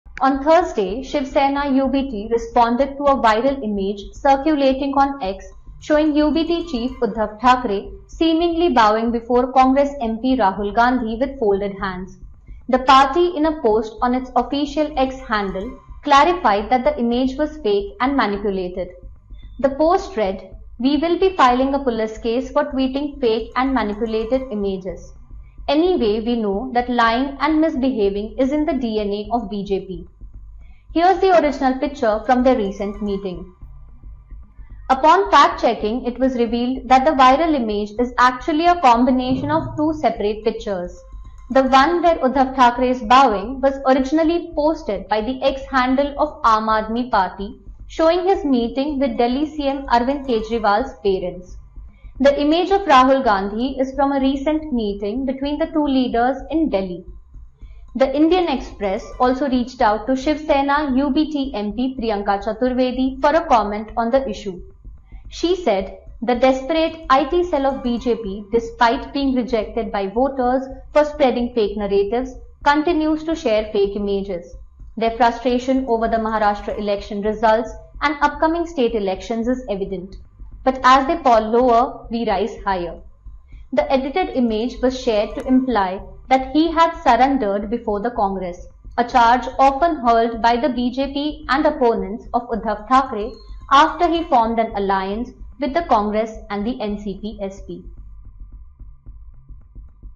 Fact-Check: Did Uddhav Thackeray Bow Before Rahul Gandhi? Here’s the Reality
A photo circulating on social media shows Shiv Sena (UTB) chief Uddhav Thackeray and Leader of Opposition in the Lok Sabha Rahul Gandhi, with the claim that Thackeray bowed down to greet the Congress leader. However, a fact-check by The Indian Express revealed that this claim is entirely false.